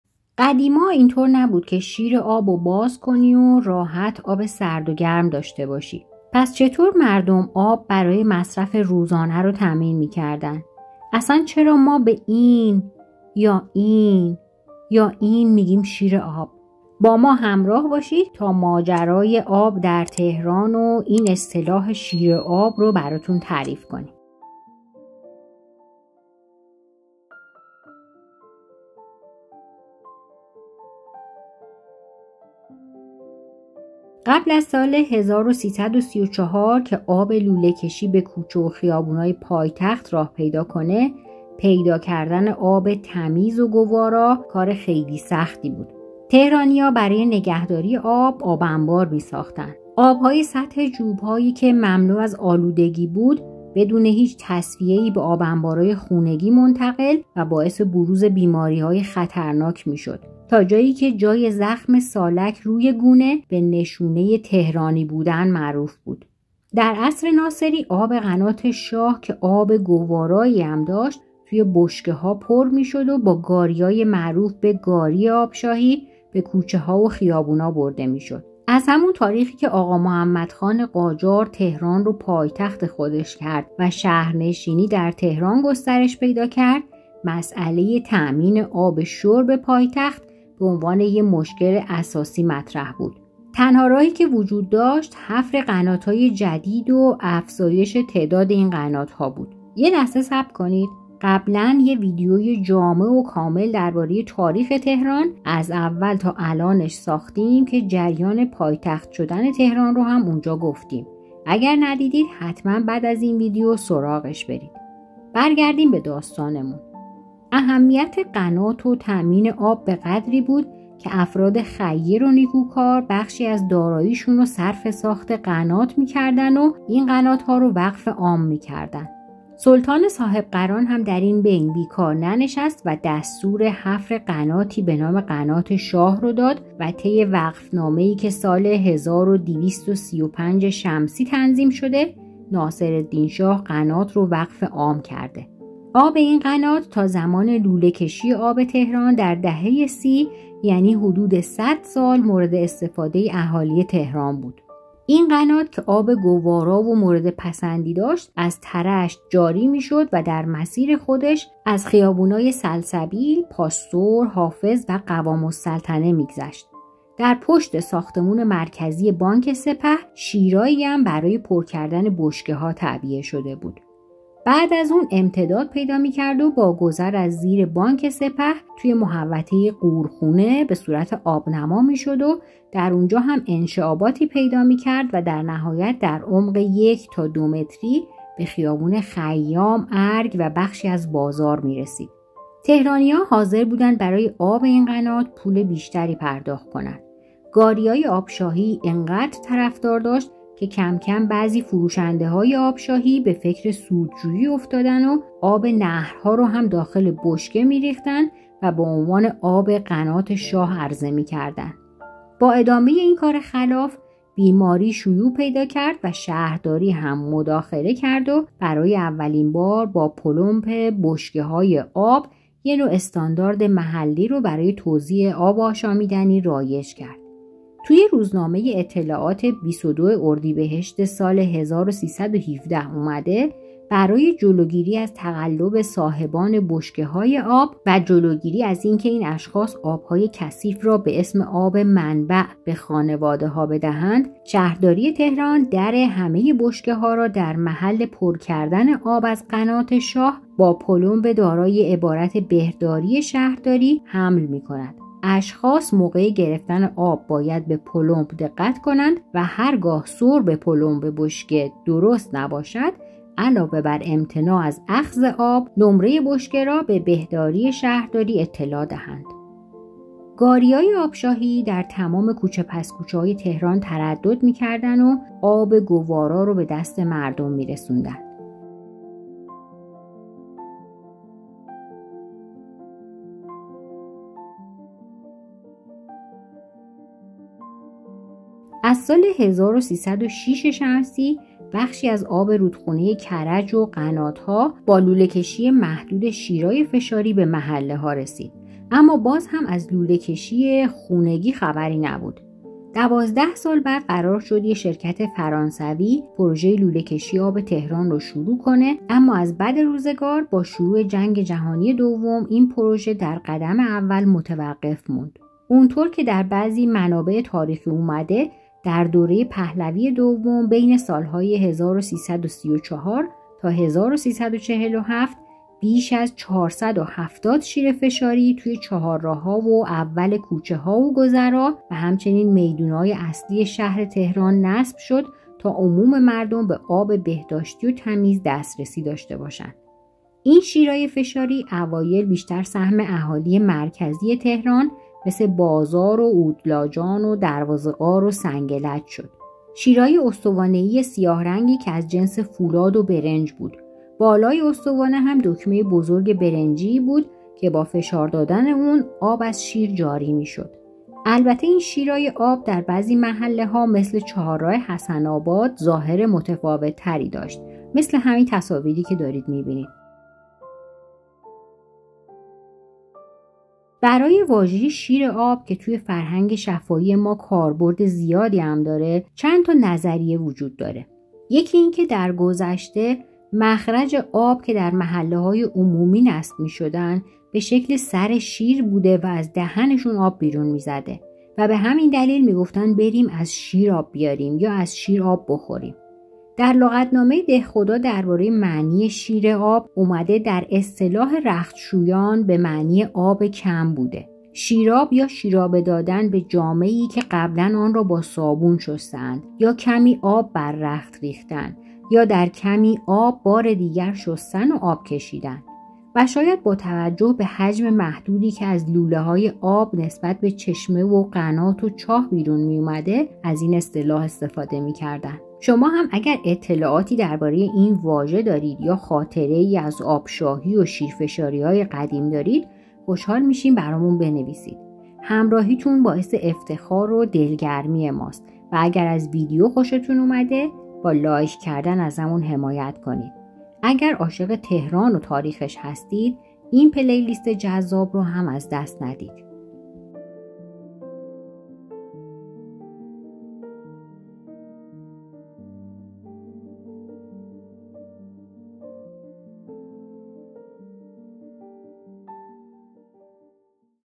از گاری آب شاهی تا شیر آب فشاری در تهران قدیم
کم آبی و بحران آب در تهران چیز جدیدی نیست. تهران تا قبل از پایتخت شدنش در زمان آغامحمدخان قاجار، نه تنها مشکل کمبود آب نداشت بلکه از روستاهای سرسبز اطراف ری بود. اما بعد از اینکه تهران پایتخت قاجاریه شد، با ازدیاد جمعیت و وسعت گرفتن محلاتش، با‌ بحران کم آبی هم مواجه شد.
اصلی ترین منبع تامین آب در تهران، قناتهای متعدد بودند که معروفترینشون قنات شاه بود و به دستور ناصرالدین شاه قاجار احداث و وقف شده بود. آب گوارای این قنات از روستای طرشت تا سلسبیل میرفت و بعد به سمت مرکز شهر سرازیر میشد. پشت ساختمان بانک سپه، جایی بود که گاری های آب شاهی بشکه های آب قنات آب شاه رو پر میکردند و برای فروش به تمام تهران میبردند. آب گاری آب شاهی خریدار زیادی در تهران داشت تا وقتی به دستور رضا شاه، با لوله کشی محدودی، شیرهای آب فشاری در مناطق مختلف تهران نصب شد و آب کرج از نهر کرج از طریق شیر آب فشاری در اختیار مردم قرار گرفت.
کم کم در دهه سی و چهل با لوله کشی آب، آب گوارا و بدون آلودگی به منازل مردم تهران رسید و مشکل آب حل شد.
اما چرا به لوله ای که آب از آن خارج می شود می گوييم شير؟ دلیل نلمگذاری شیر آب چیست؟
در این ویدیو درباره تاریخچه آب در تهران براتون‌گفتیم.
اگر از این ویدیو خوشتون اومد، برای دیدن ویدیوهای بیشتر، روی لینک زیر بزنید: